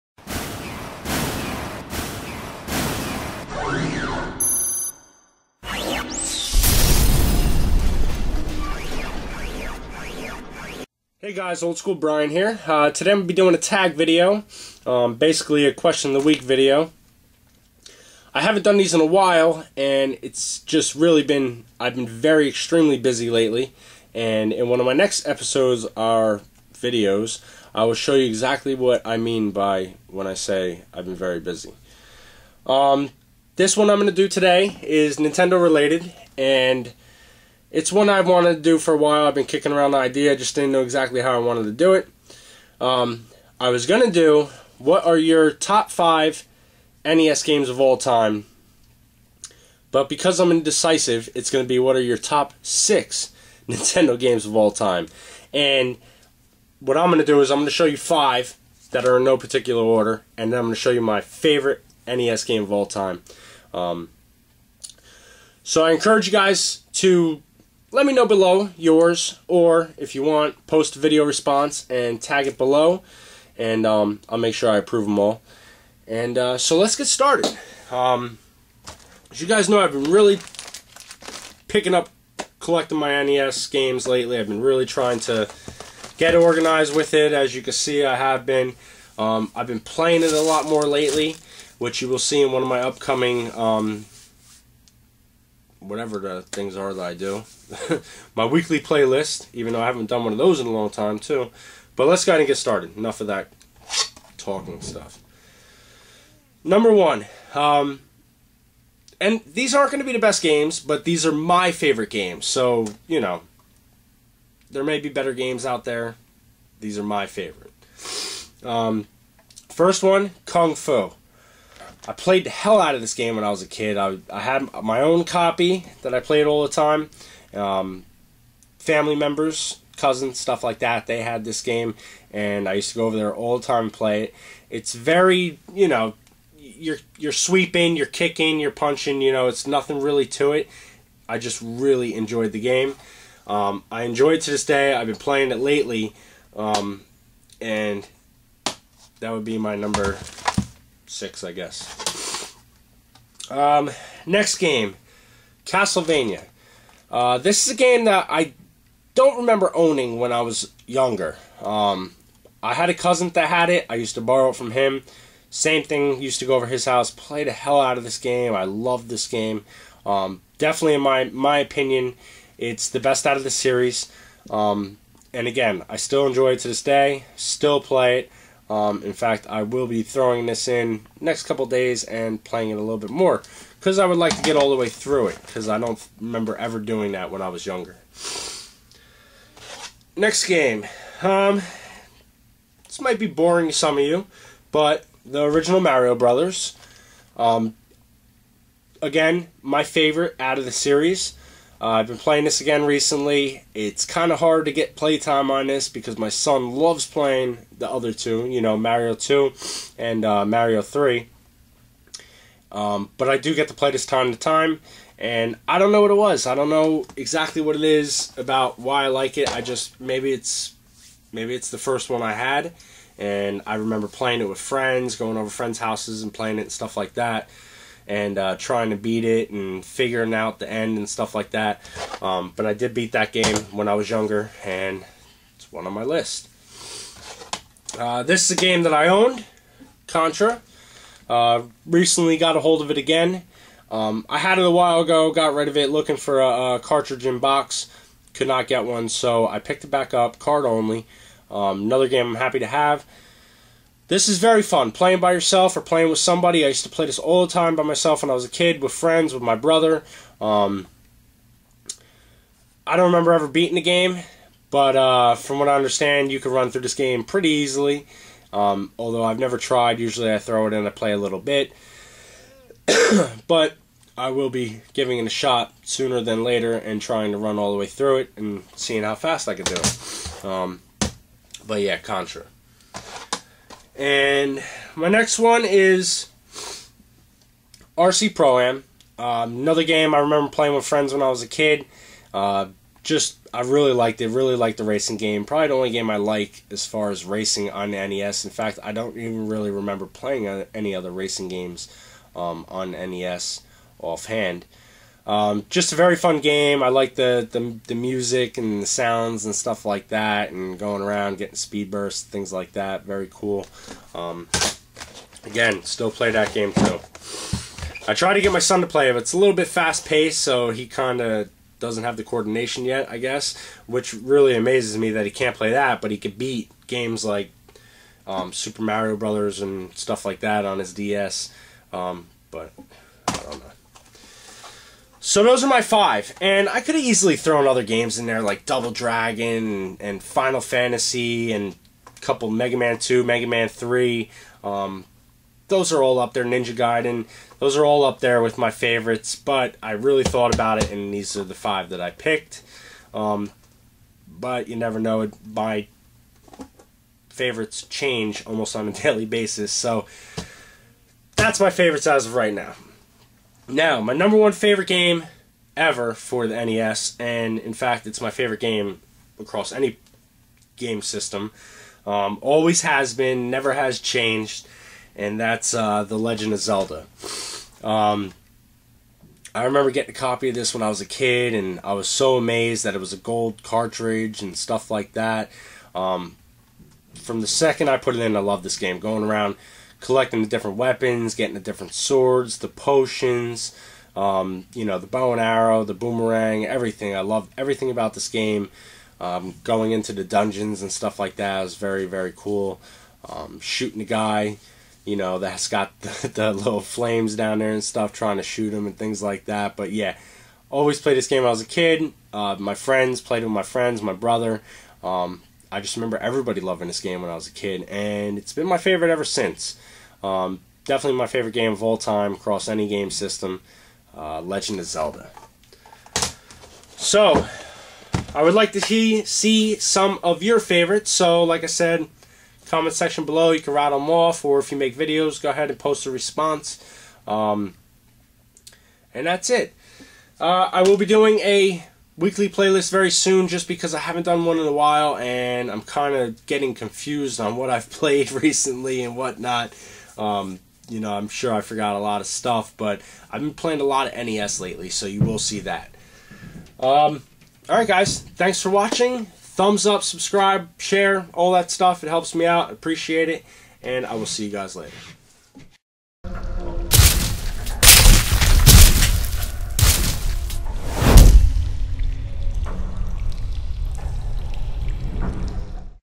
Tag: Question of the Week. What are your Top 6 NES games of all time?
A Weekly Question.. What are your top 6 NES Games of all time?
Post responses below.

If you enjoy my channel make sure to BOX it.

We have tons of different sprites.